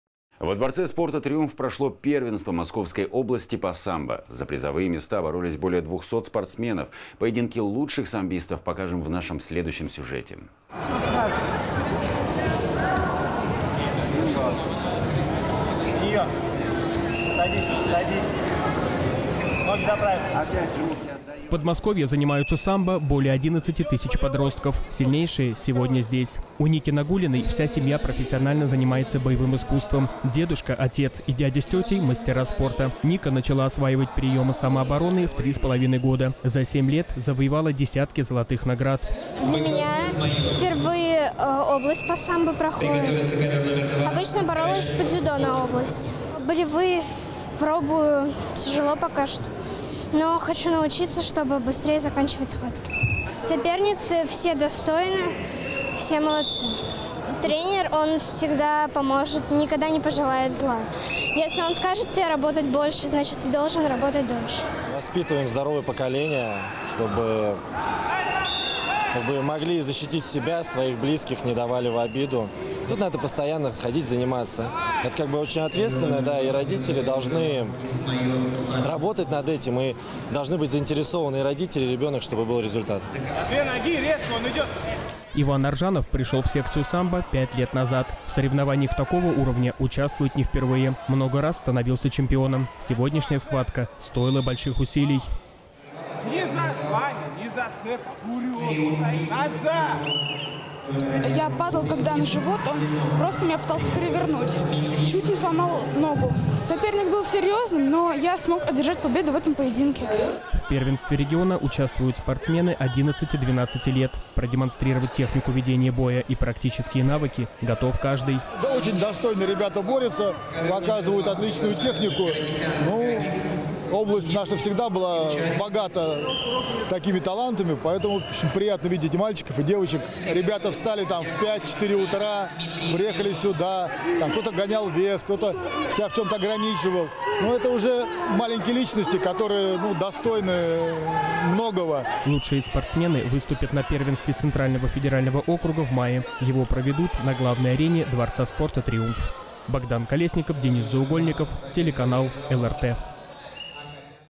Первенство Московской области по самбо
Во Дворце спорта «Триумф» прошло первенство Московской области по самбо. За призовые места боролись более 200 спортсменов. Поединки лучших самбистов покажем в нашем следующем сюжете.

В Подмосковье самбо занимаются более 11 000 подростков. Сильнейшие сегодня здесь. У Ники Нагулиной вся семья профессионально занимается боевым искусством. Дедушка, отец и дядя с тётей – мастера спорта. Ника начала осваивать приёмы самообороны в 3,5 года. За семь лет завоевала десятки золотых наград.

Ника Нагулина, участница первенства Московской области по самбо: «Для меня впервые область по самбо проходит. Обычно я боролась по дзюдо на область. Болевые пробую, тяжело пока что, но хочу научиться, чтобы быстрее заканчивать схватки. Соперницы все достойны, все молодцы. Тренер, он всегда поможет, никогда не пожелает зла. Если скажет тебе работать больше, значит, ты должен работать дольше».

Дмитрий Нагулин, тренер по самбо СШОР г.о. Можайск, отец Ники Нагулиной: «Воспитываем здоровое поколение, чтобы могли защитить себя, своих близких не давали в обиду. Тут надо постоянно ходить заниматься. Это очень ответственно. Родители должны работать над этим. И должны быть заинтересованы и родители, и ребёнок, чтобы был результат».

Иван Аржанов пришёл в секцию самбо пять лет назад. В соревнованиях такого уровня участвует не впервые. Много раз становился чемпионом. Сегодняшняя схватка стоила больших усилий.

Иван Аржанов, участник первенства Московской области по самбо: «Я падал когда на живот, он просто меня пытался перевернуть, чуть не сломал ногу. Соперник был серьёзный, но я смог одержать победу в этом поединке».

В первенстве региона участвуют спортсмены 11-12 лет. Продемонстрировать технику ведения боя и практические навыки готов каждый.

Сергей Фомкин, президент федерации самбо Московской области: «Очень достойно ребята борются, показывают отличную технику. Область наша всегда была богата такими талантами, поэтому приятно видеть и мальчиков, и девочек. Ребята встали в 4-5 утра, приехали сюда. Кто-то гонял вес, кто-то себя в чём-то ограничивал. Ну, это уже маленькие личности, которые достойны многого».

Лучшие спортсмены выступят на Первенстве Центрального федерального округа в мае. Его проведут на главной арене Дворца спорта «Триумф».